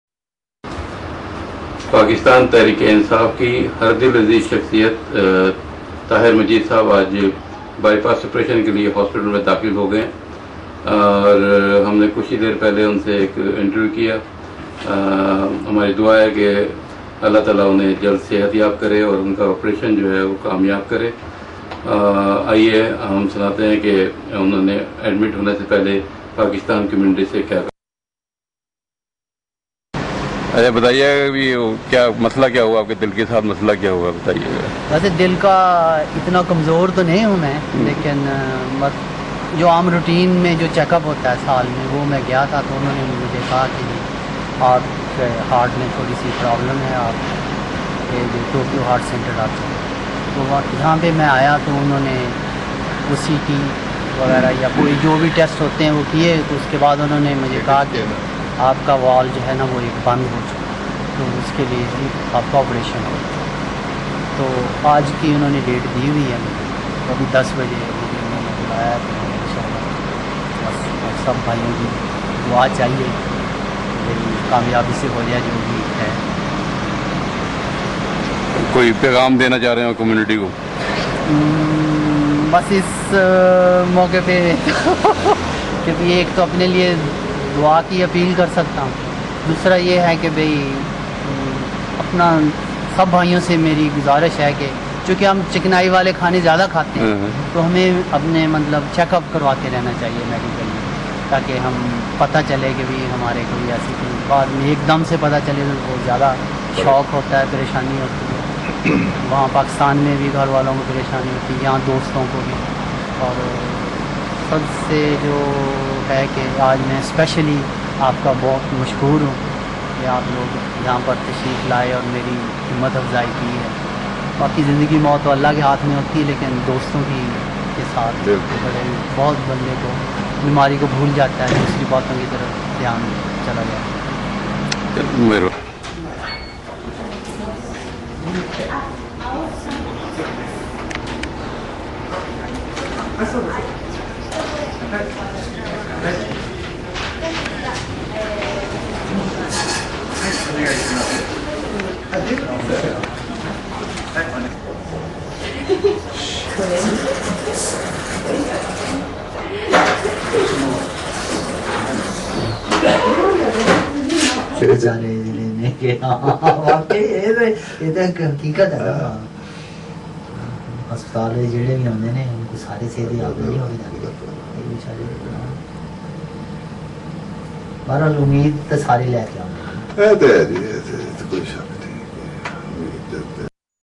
TAHIR MUJEED ADMITED IN HOSPITAL FOR HEART BY PASS
TAHIR MUJEED ADMITED IN HOSPITAL FOR HEART BY PASS 12/08-2011